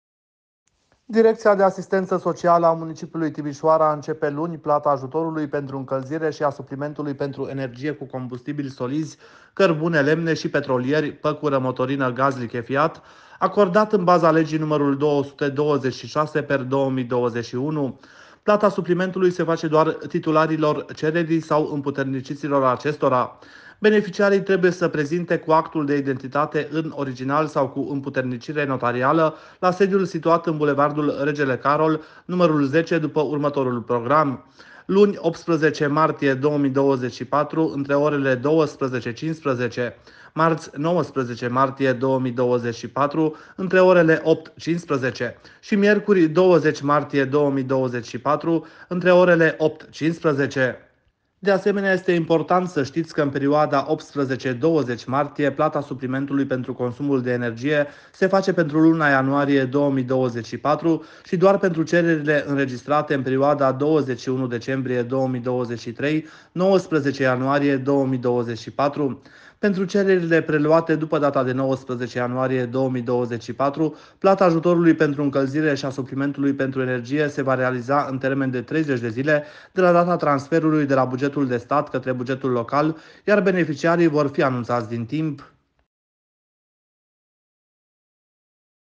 SE DAU BANII PENTRU AJUTORUL DE ÎNCĂLZIRE, LA TIMIȘOARA
Directia de Asistenta Sociala a Municipiului Timisoara 𝐢̂𝐧𝐜𝐞𝐩𝐞 𝐥𝐮𝐧𝐢 𝐩𝐥𝐚𝐭𝐚 𝐚𝐣𝐮𝐭𝐨𝐫𝐮𝐥𝐮𝐢 𝐩𝐞𝐧𝐭𝐫𝐮 𝐢̂𝐧𝐜𝐚̆𝐥𝐳𝐢𝐫𝐞 𝐬̦𝐢 𝐚 𝐬𝐮𝐩𝐥𝐢𝐦𝐞𝐧𝐭𝐮𝐥𝐮𝐢 𝐩𝐞𝐧𝐭𝐫𝐮 𝐞𝐧𝐞𝐫𝐠𝐢𝐞 𝐜𝐮 𝐜𝐨𝐦𝐛𝐮𝐬𝐭𝐢𝐛𝐢𝐥𝐢 solizi (cărbune, lemne) şi petrolieri (păcură, motorină, gaz lichefiat), acordat în baza Legii nr.226/2021. Plata suplimentului se face doar titularilor cererii sau împuterniciților acestora. Beneficiarii trebuie să se prezinte cu actul de identitate în original sau cu împuternicire notarială, la sediul situat în bd. Regele Carol I nr.10, după următorul program:
𝐋𝐮𝐧𝐢, (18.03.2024), între orele 𝟏𝟐:𝟎𝟎-𝟏𝟓:𝟎𝟎
𝐌𝐚𝐫𝐭̦𝐢, (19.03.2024), între orele 𝟎𝟖:𝟎𝟎-𝟏𝟓:𝟎𝟎
𝐌𝐢𝐞𝐫𝐜𝐮𝐫𝐢, (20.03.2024), între orele 𝟎𝟖:𝟎𝟎-𝟏𝟓:𝟎𝟎